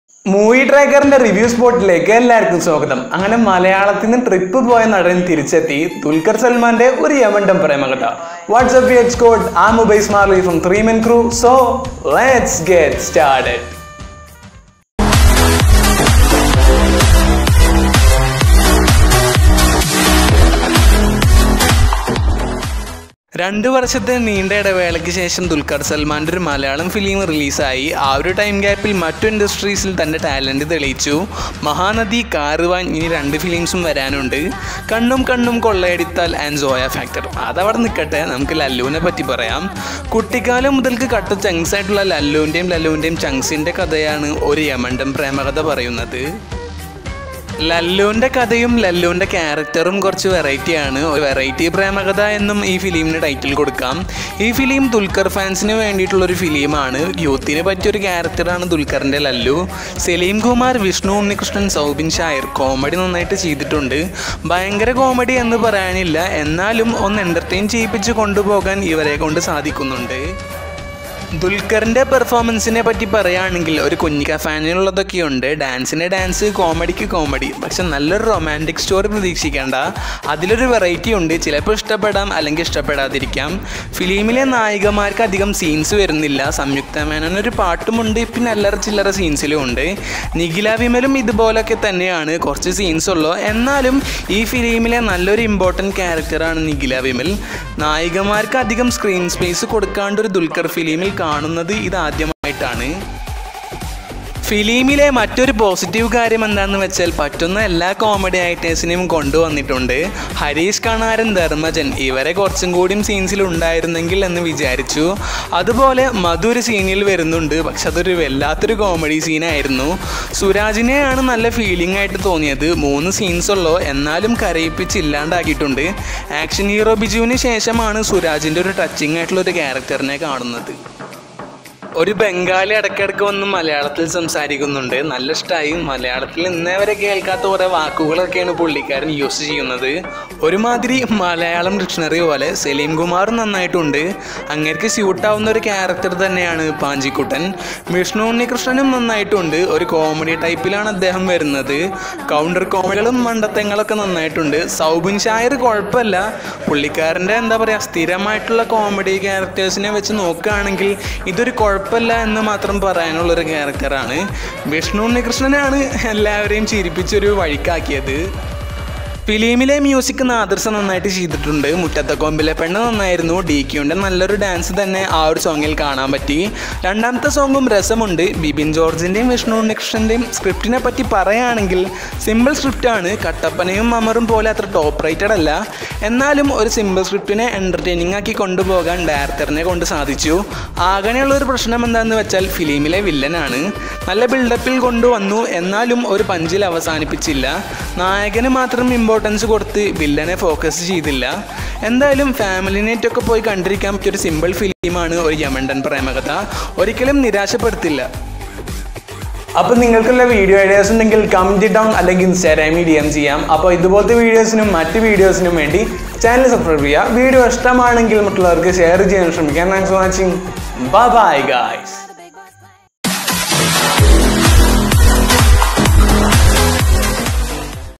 Oru Yamandan Premakadha Movie Review By Ubais Marly | Dulquer Salman | Movie Tracker | 3Men Crew
This Video Is Not Created To Insult or Disrespect Anyone Personally

The Copyright Of The Images Used In The Video Doesn't Belongs To Me . IT Belongs To Respected Copyrighted Owner